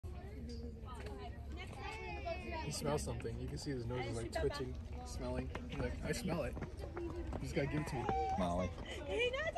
The Good Life of a Grand Canyon Squirrel
Squirrels are the most commonly sighted wildlings it seems, and unfazed by curious tourists.